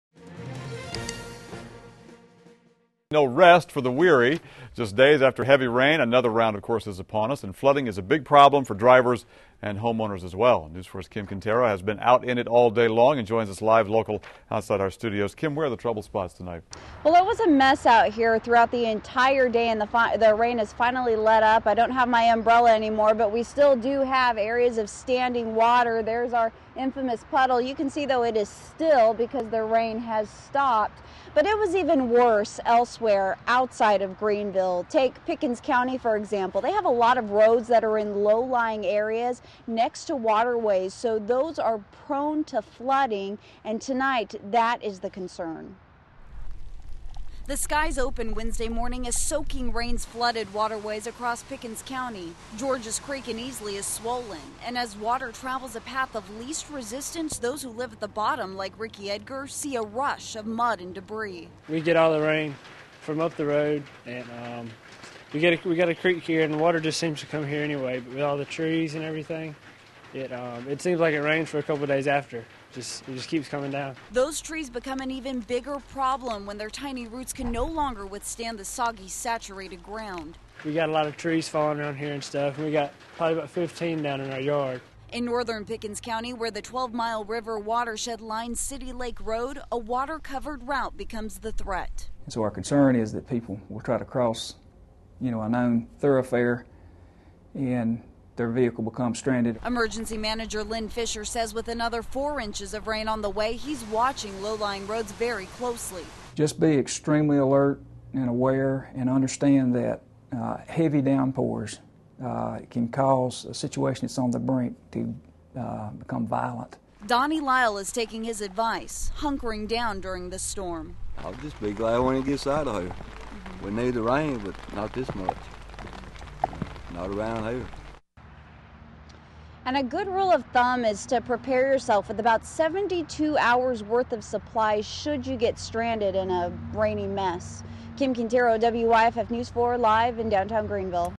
Heavy Rain Fills Waterways, Threatens Roads
The skies opened Wednesday morning as soaking rains flooded waterways across Pickens County.